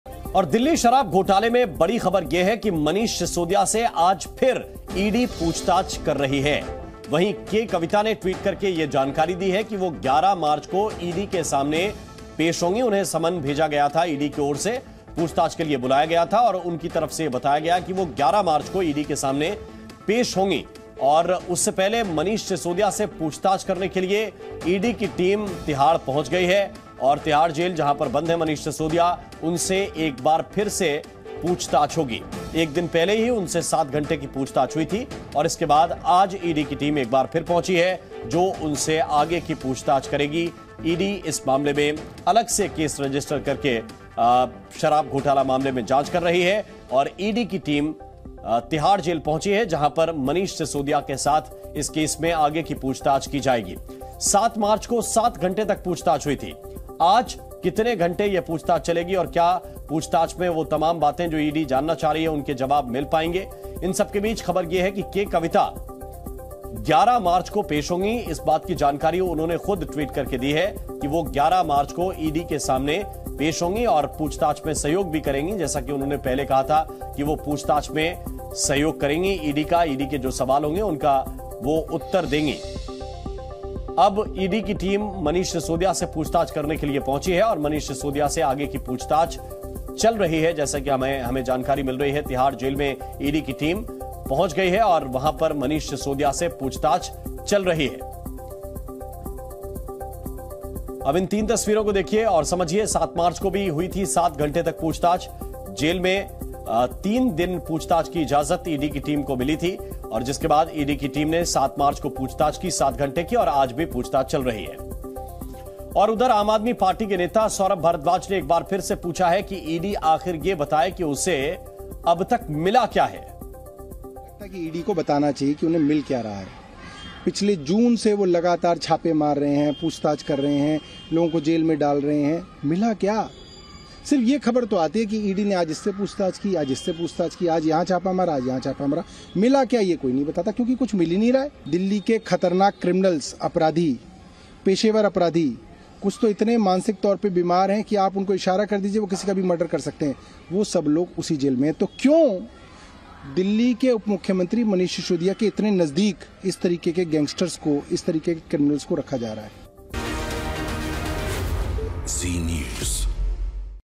Delhi Liquor Scam: Manish Sisodia से ED पूछताछ पर Saurabh Bhardwaj ने उठाया सवाल, 'अब तक क्या मिला?'
दिल्ली शराब नीति घोटाले मामले में मंगलवार को हुई पूछताछ के बाद आज फिर डिप्टी सीएम मनीष सिसोदिया से पूछताछ करने तिहाड़ जेल पहुंची है ED की टीम। इसको लेकर आम आदमी पार्टी के नेता सौरभ भारद्वाज ने सवाल उठाया है और कहा है कि, 'ED ये बताए कि अब तक पूछताछ में क्या मिला?'.

After the interrogation on Tuesday in Delhi Liquor Policy Scam case, ED team has again reached Tihar Jail to interrogate Deputy CM Manish Sisodia. Commenting on this, Aam Aadmi Party leader Saurabh Bhardwaj has raised a question and said, 'ED should tell what has been found in the investigation so far?'.